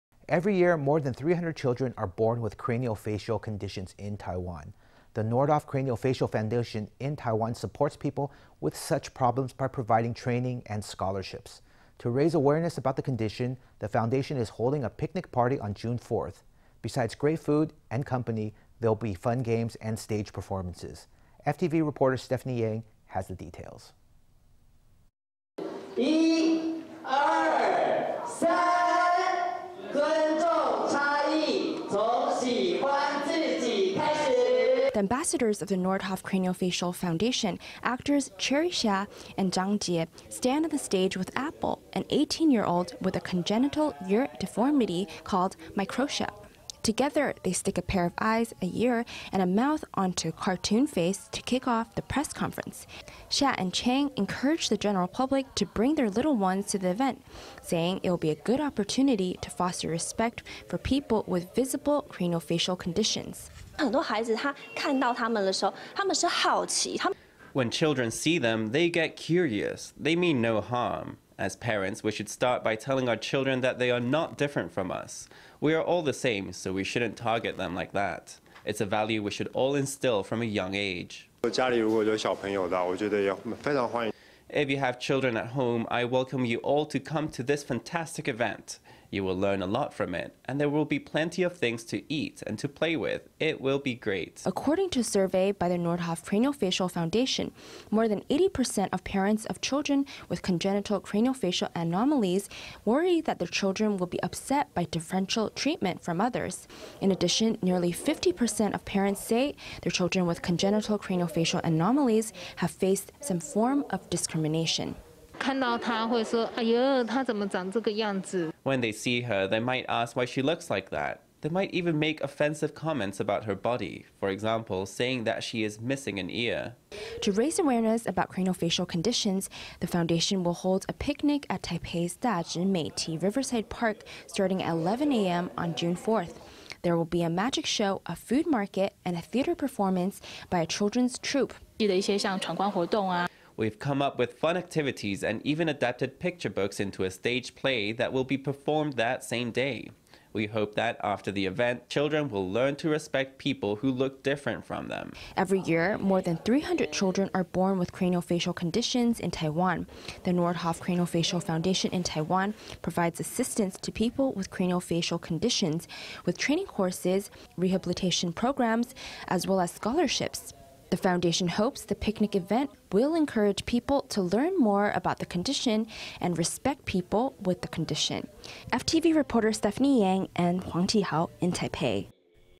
Foundation organizes riverside picnic to raise awareness of craniofacial conditions
Every year, more than 300 children are born with craniofacial conditions in Taiwan. The Noordhoff Craniofacial Foundation in Taiwan supports people with such problems by providing training and scholarships. To raise awareness about the condition, the foundation is holding a picnic party on June 4. Besides great food and company, there’ll also be fun games and stage performances. FTV reporter Stephany Yang has the details.

The ambassadors of the Noordhoff Craniofacial Foundation, actors Cherry Hsia and Chang Chieh, stand on the stage with Apple, an 18-year-old with a congenital ear deformity called microtia. Together, they stick a pair of eyes, an ear, and a mouth onto a cartoon face to kick off the press conference. Hsia and Chang encouraged the general public to bring their little ones to the event, saying it will be a good opportunity to foster respect for people with visible craniofacial conditions.

Cherry Hsia
Ambassador
When children see them, they get curious. They mean no harm. As parents, we should start by telling our children that they are not different from us. We are all the same, so we shouldn’t target them like that. It’s a value we should all instill from a young age.

Chang Chieh
Ambassador
If you have children at home, I welcome you all to come to this fantastic event. You will learn a lot from it, and there will be plenty of things to eat and to play with. It will be great.

According to a survey by the Noordhoff Craniofacial Foundation, more than 80% of parents of children with congenital craniofacial anomalies worry that their children will be upset by differential treatment from others. In addition, nearly 50% of parents say their children with congenital craniofacial anomalies have faced some form of discrimination.

Apple’s mother
When they see her, they might ask why she looks like that. They might even make offensive comments about her body, for example saying that she is missing an ear.

To raise awareness about craniofacial conditions, the foundation will hold a picnic party at Taipei’s Dazhi Meiti Riverside Park starting at 11 a.m. on June 4. There will be a magic show, a food market, and a theater performance by a children’s troupe.

Chen I-ling
Noordhoff Craniofacial Foundation
We’ve come up with fun activities and even adapted picture books into a stage play that will be performed that same day. We hope that, after the event, children will learn to respect people who look different from them.

Every year, more than 300 children are born with craniofacial conditions in Taiwan. The Noordhoff Craniofacial Foundation in Taiwan provides assistance to people with craniofacial conditions, with training courses, rehabilitation programs, as well as scholarships. The foundation hopes the picnic event will encourage people to learn more about the condition and respect people with the condition.